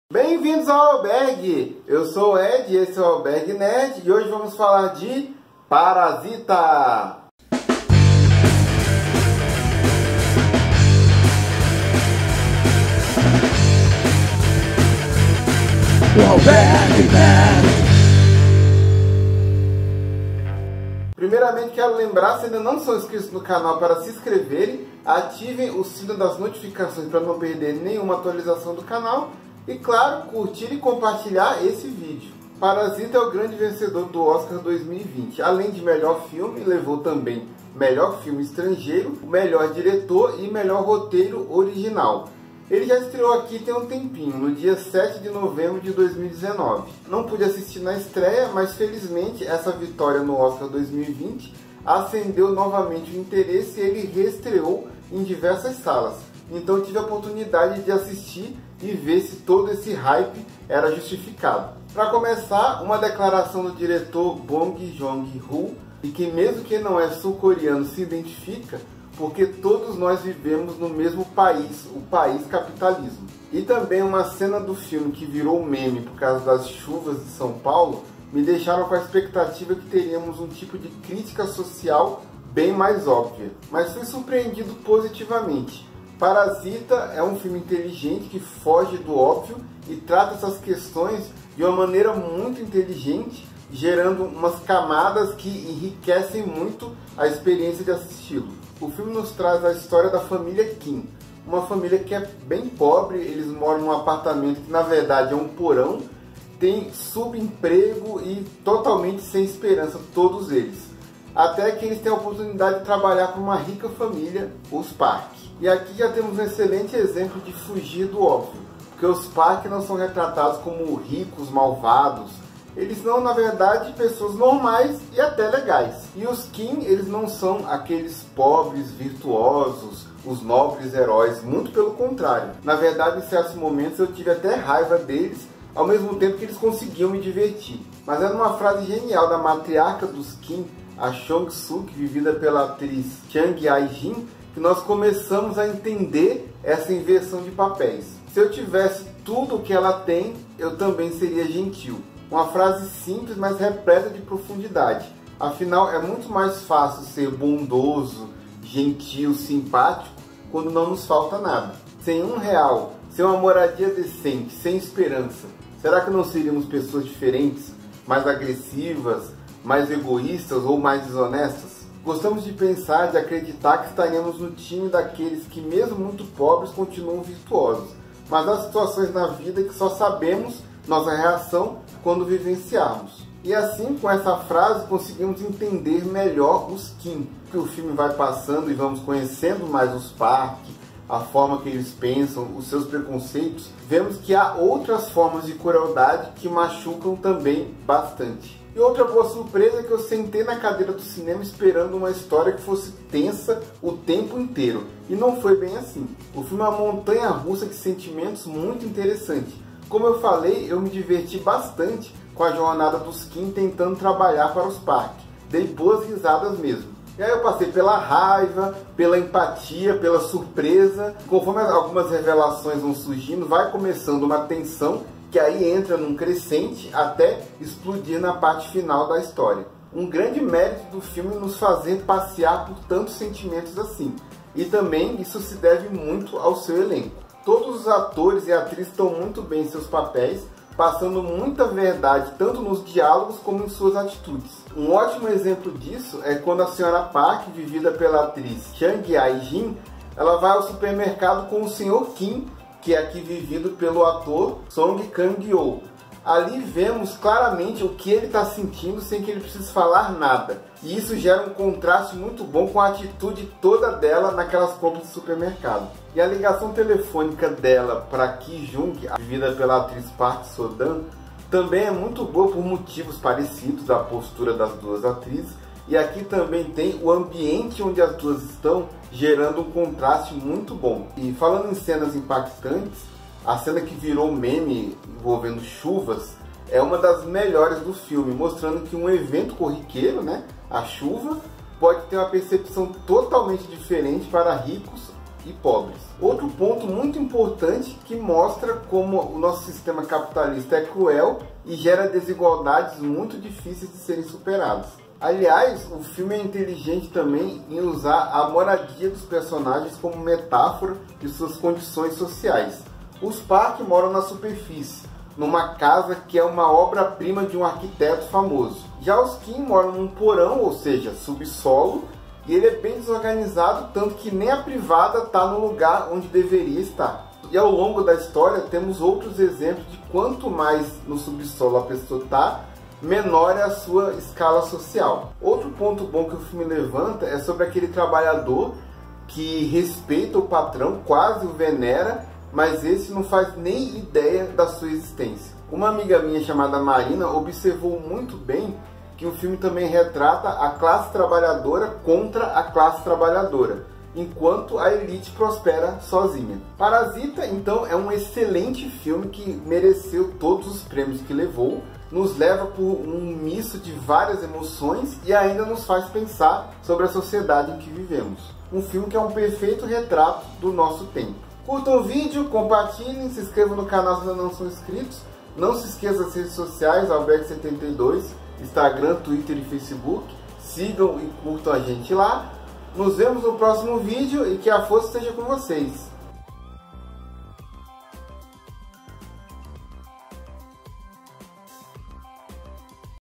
Parasita | Review Sem Spoilers
Toda a família Kim está desempregada, vivendo num porão sujo e apertado. Eles conseguem trabalho com a família Park, que tem uma vida completamente oposta a deles. Será que esses mundos tão diferentes podem conviver em harmonia ou as barreiras sociais entre eles são intransponíveis?

Essa é a trama de Parasita, filme sul-coreano que foi o grande vencedor do Oscar 2020 e, no vídeo de hoje, vamos falar um pouco desse grande sucesso de 2019 que ganhou novo fôlego esse ano. É realmente tão bom assim?